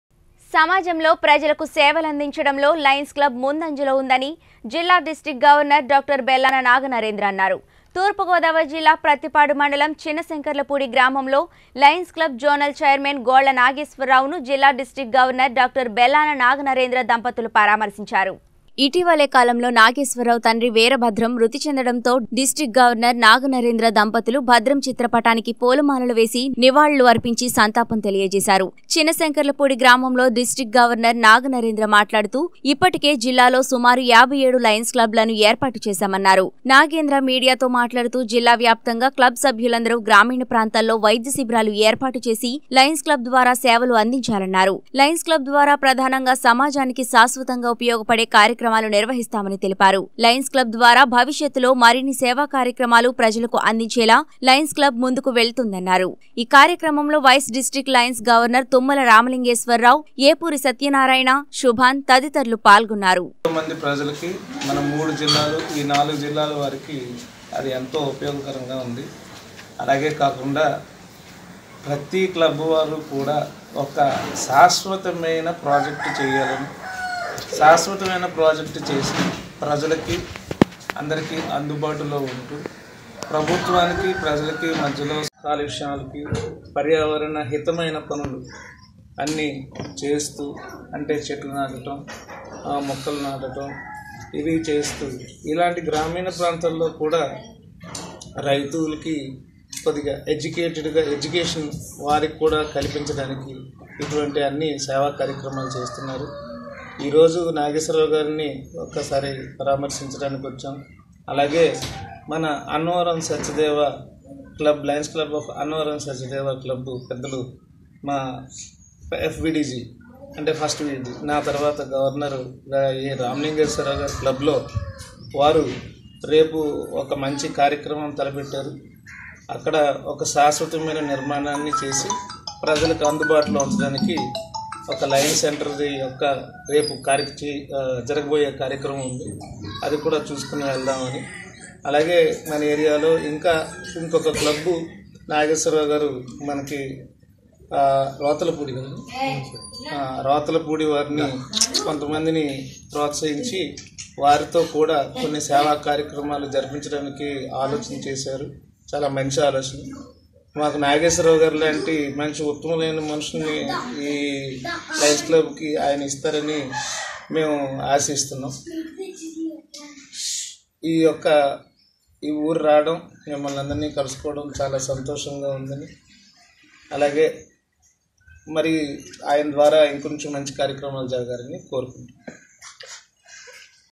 లయన్స్ క్లబ్ సేవలను విస్తృతం చేస్తాం జిల్లా దృష్టికి గవర్నర్ డాక్టర్ బెల్లం నాగ నరేంద్ర ప్రసాద్
లయన్స్ క్లబ్ సేవలను విస్తృతం చేస్తాం జిల్లా దృష్టికి గవర్నర్ డాక్టర్ బెల్లం నాగ  నరేంద్ర ప్రసాద్ - తూర్పు గోదావరి జిల్లా | 19 Newstelugu